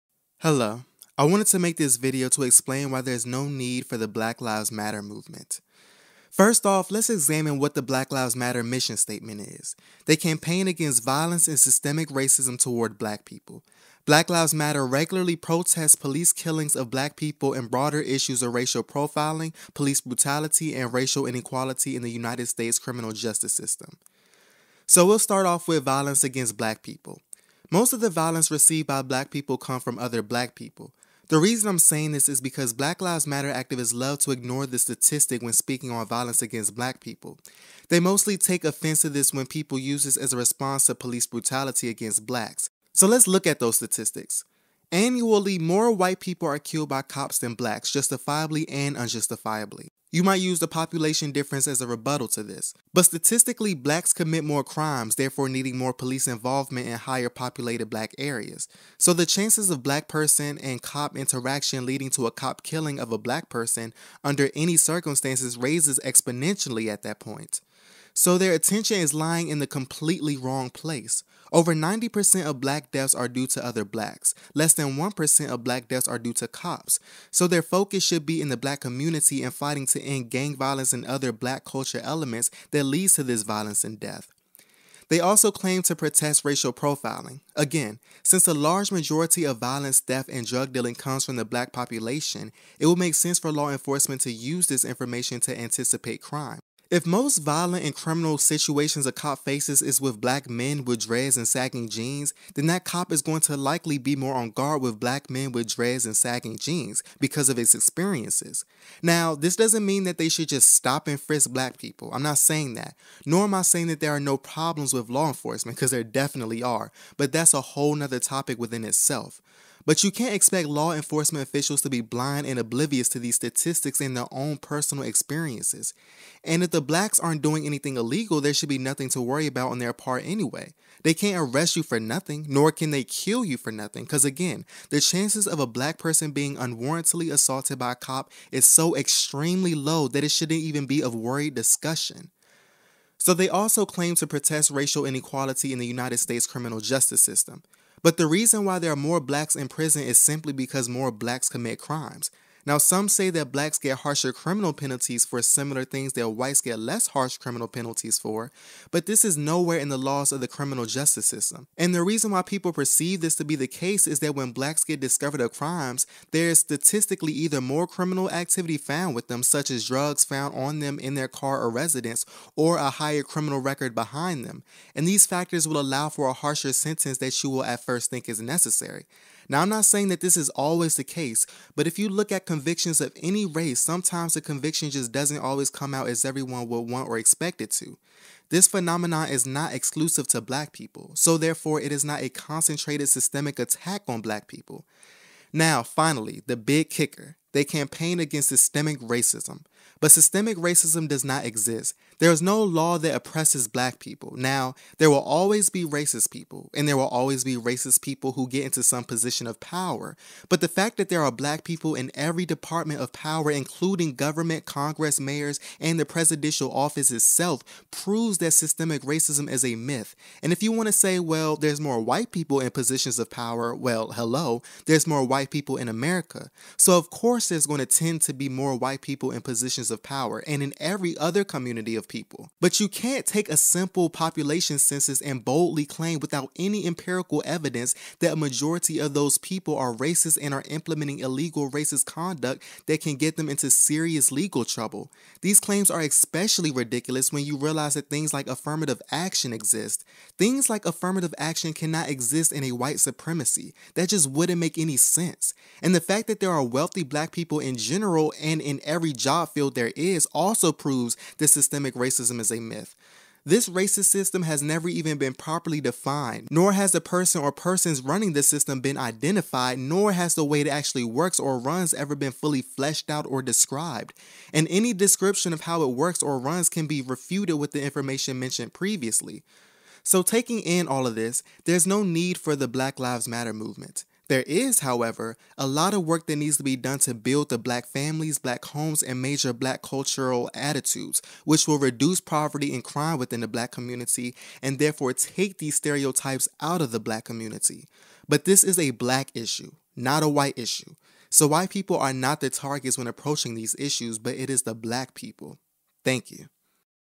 Black Lives Matter Movement Does Not Need to Exist
Explanation of the irrelevence of the black lives matter movement.